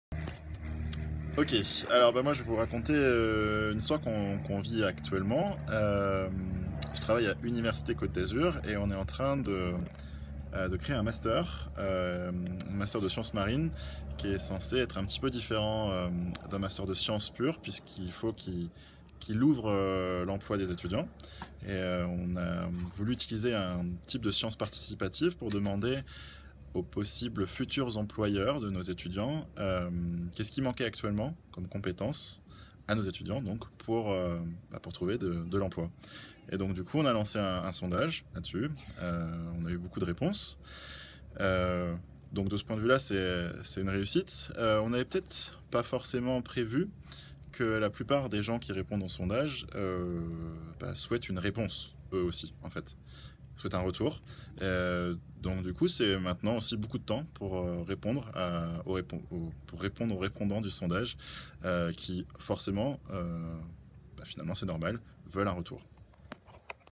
Atelier Valeurs & Participation, Pays d'Azur 14 10 2017
Vidéo réalisée par les participants le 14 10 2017 en Pays d'Azur, avec le  soutien du CPIE des îles de Lérins et du Pays d'Azur, dans le cadre du projet de recherche-action participative "ScVal - Sciences et Valeurs dans la participation".